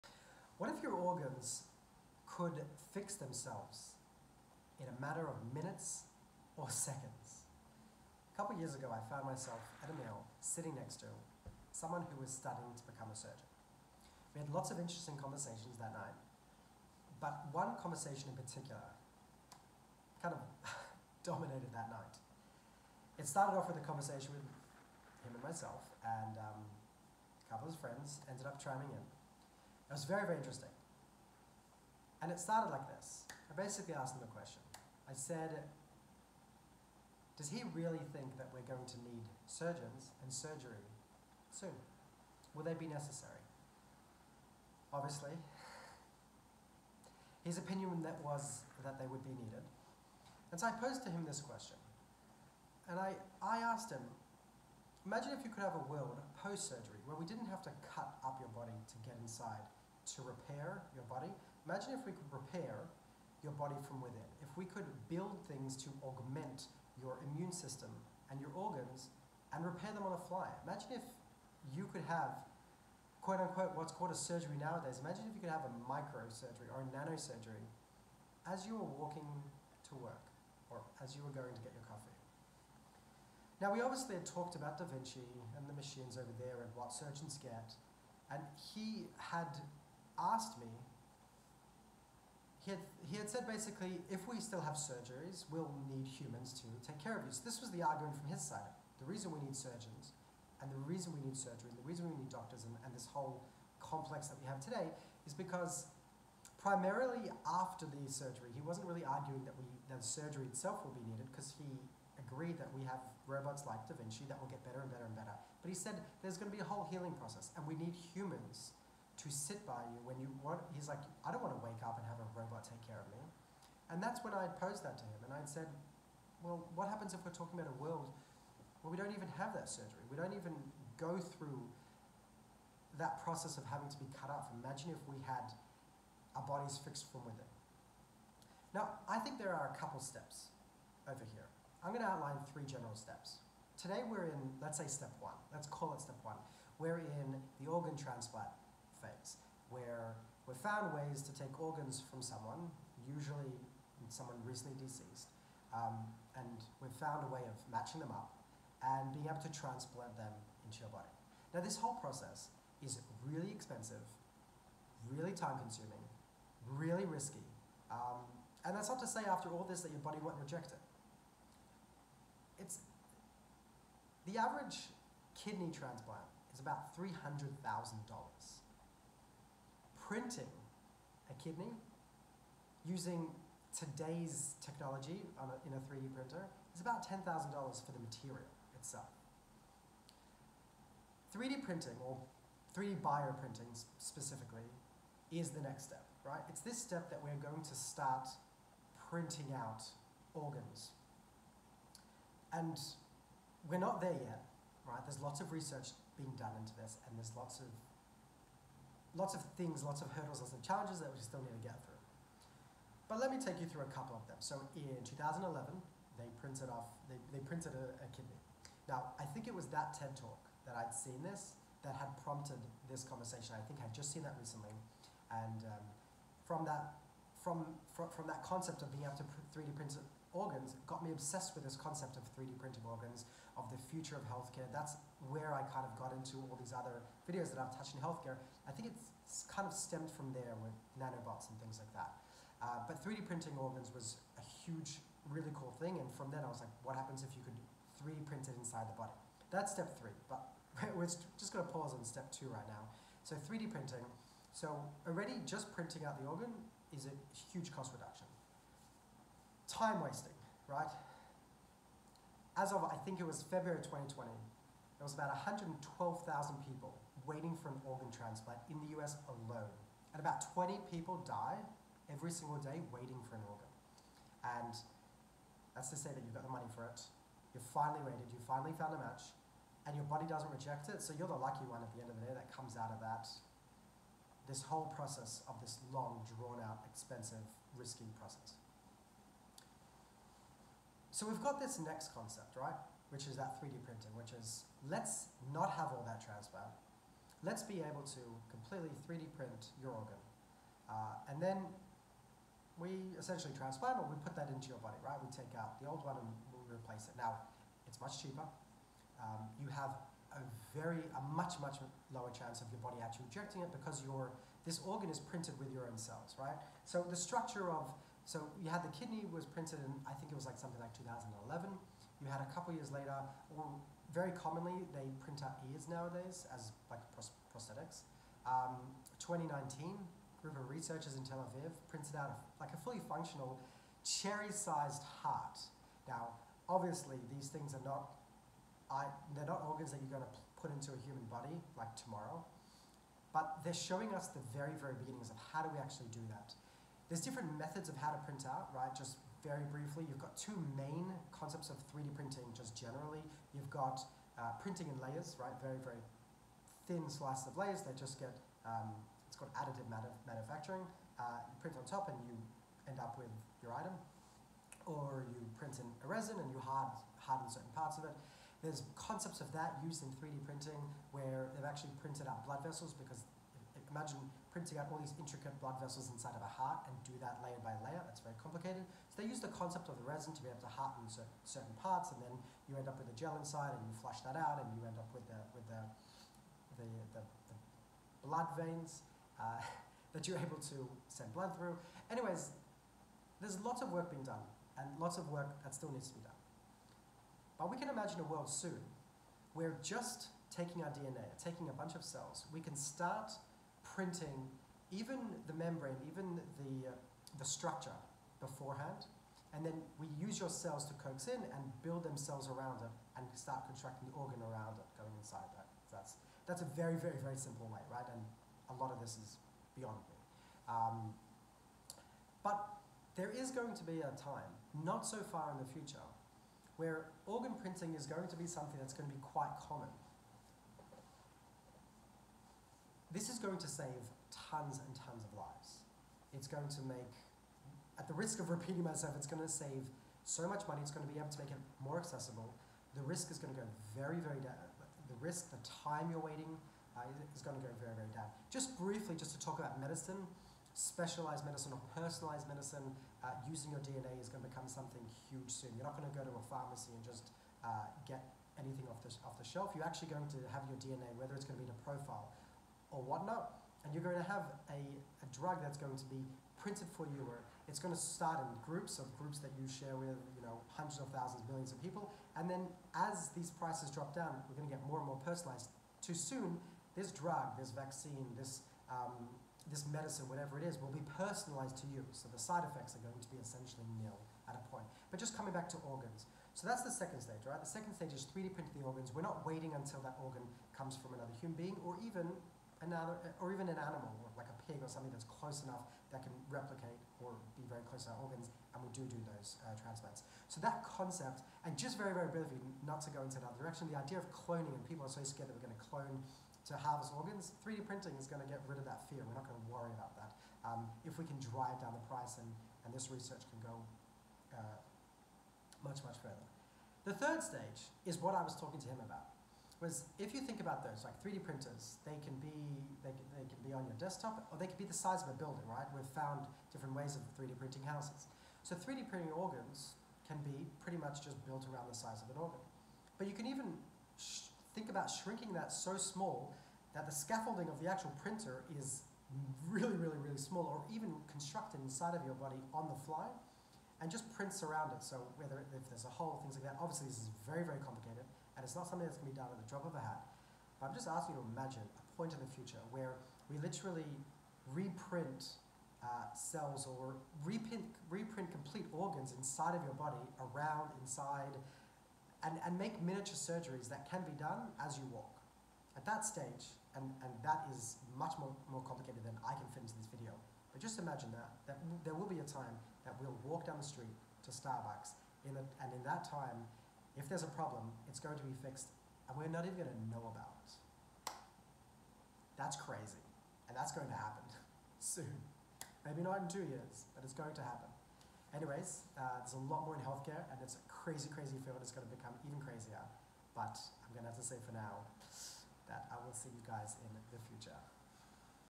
Day 28: 3D Printed Organs
Will 3Dprinted organs and other breakthroughs mark the death of surgery?

Do you want to create an AR product/app and need some help with product design and understanding this paradigm-shifting field?